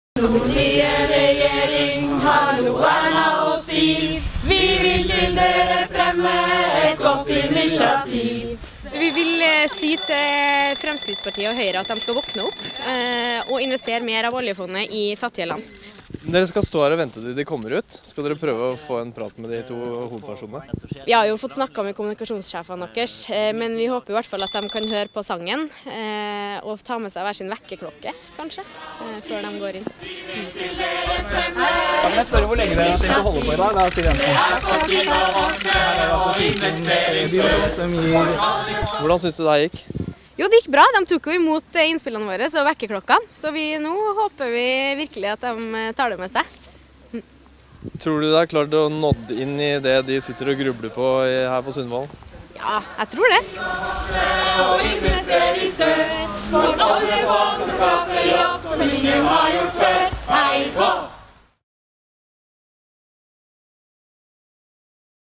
«Alle» vil ha et ord med i regjeringserklæringen
Først kom Stine Sofies stiftelse. Så overtok Changemaker og Kirkens nødhjelp jobben med på påvirke det pågående arbeidet med en regjeringserklæring på Sundvollen. I hvert fall forsøket med å påvirke.

Changemaker,Erna Solberg,Kirkens Nødhjelp,regjering,regjeringserklæring,siv jensen,stine sofies stiftelse,Sundvollen,Nyheter,nyheter,Videoer